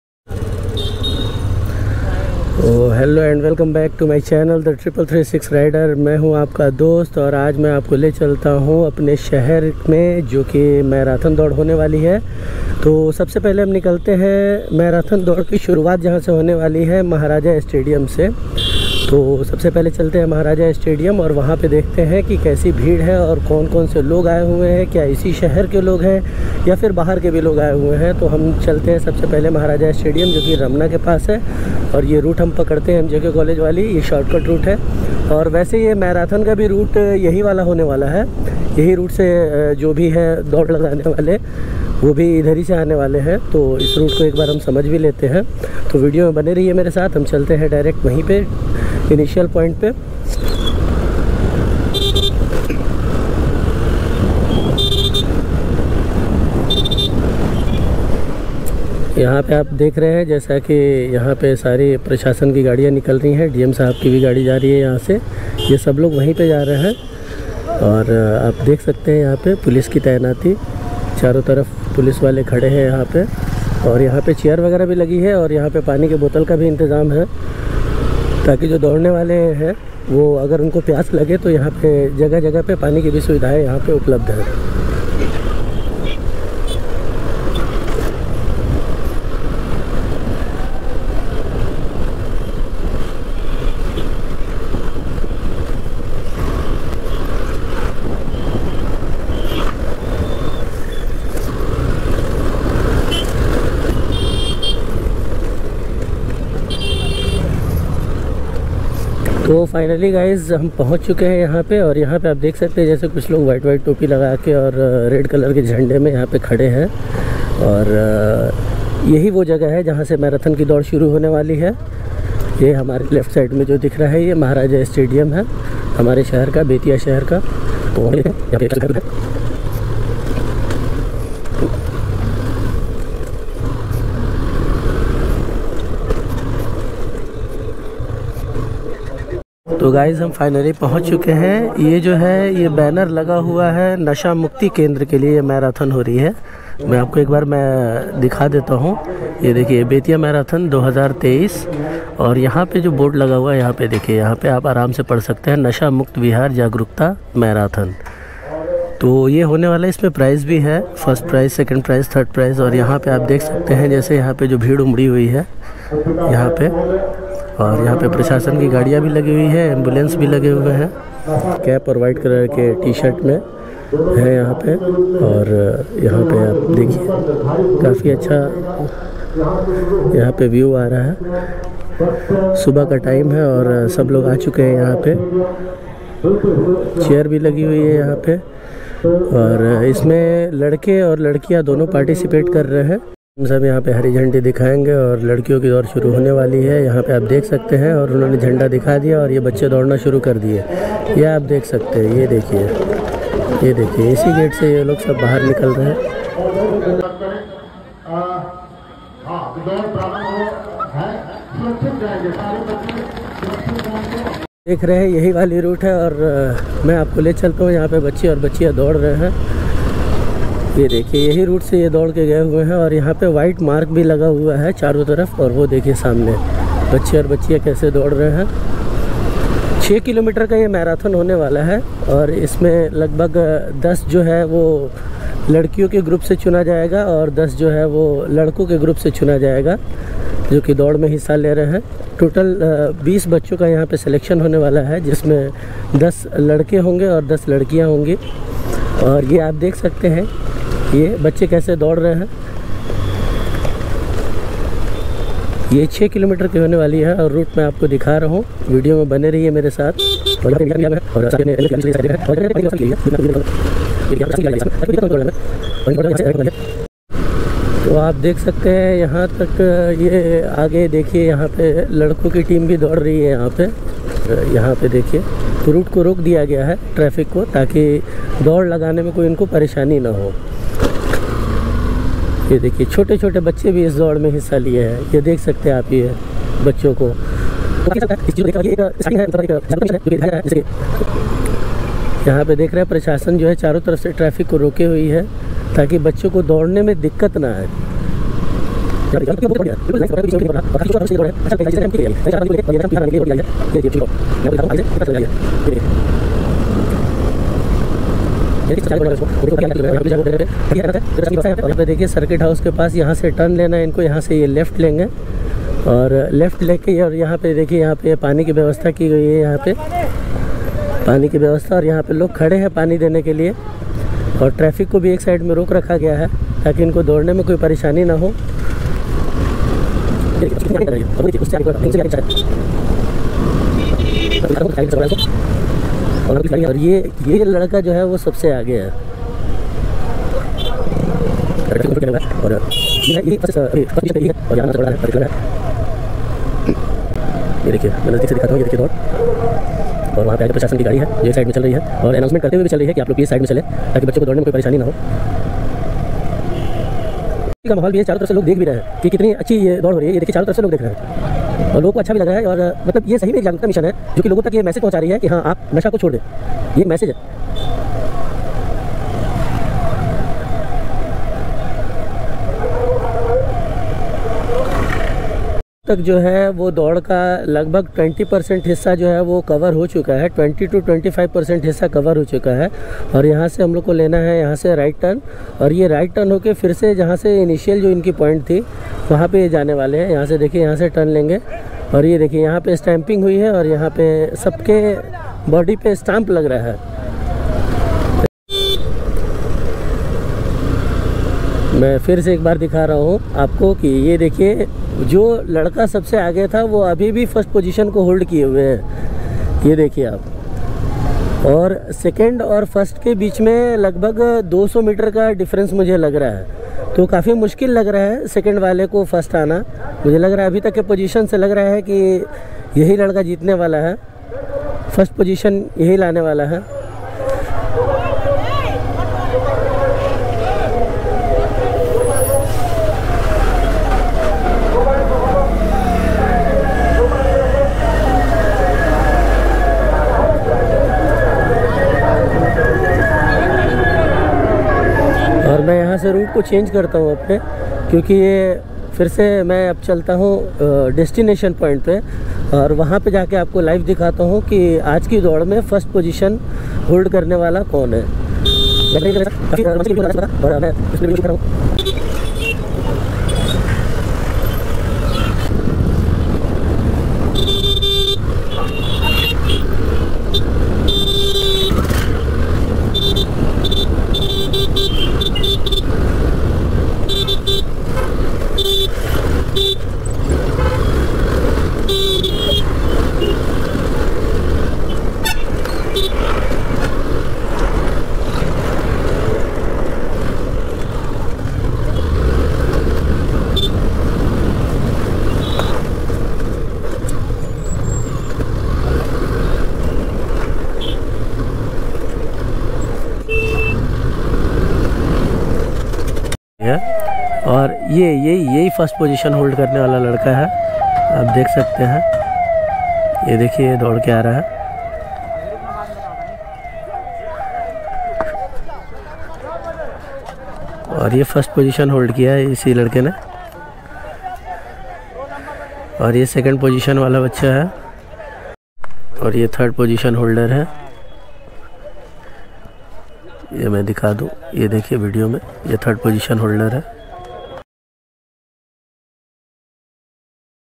Marathon at Bettiah 2023
This Marathon is organised to create awareness among people Who is habitual of drinks,
Be healthy and be strong
Say No to Alcohol and Alcoholic